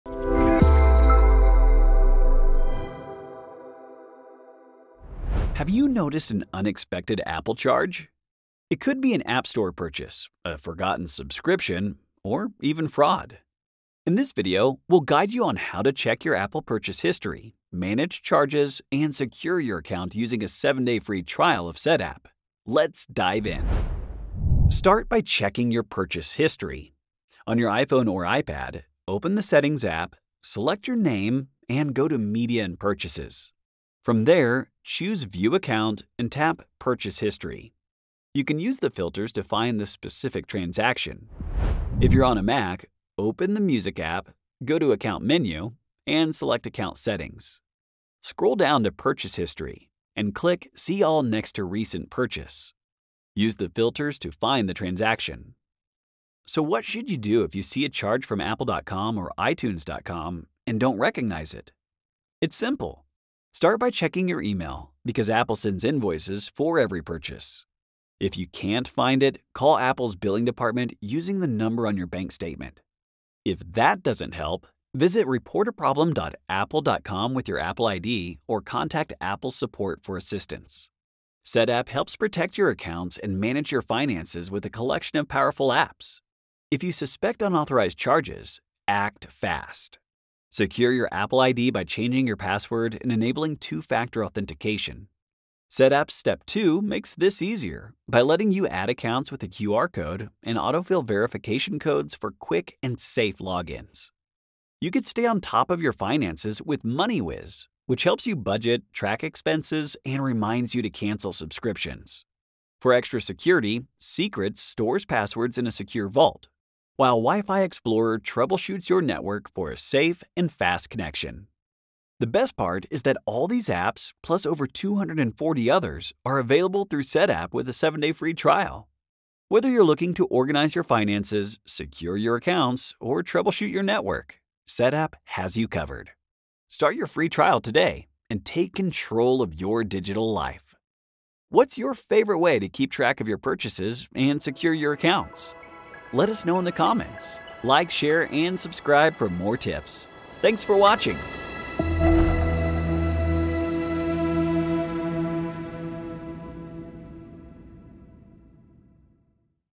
How to Check Unexpected Apple Bill Charges
🤔 Noticed unexpected Apple bill charges? Whether it’s an App Store purchase, forgotten subscription, or fraud, it’s important to know how to check and manage your Apple purchase history. In this video, we’ll walk you through checking your Apple charges on iPhone, iPad, and Mac, and show you how to secure your account and manage your finances with smart Setapp apps.

✨ Timestamps:
00:00 – Intro
00:24 – How to check Apple purchase history on iPhone and iPad
00:42 – How to check purchase history on Mac
00:56 – What to do if you don’t recognize a charge
01:21 – Secure your Apple ID with Step Two
01:44 – Manage spending with MoneyWiz
01:52 – Use Secrets for passwords and WiFi Explorer for connectivity issues

💬 How do you track your subscriptions and digital purchases? Drop your tips in the comments!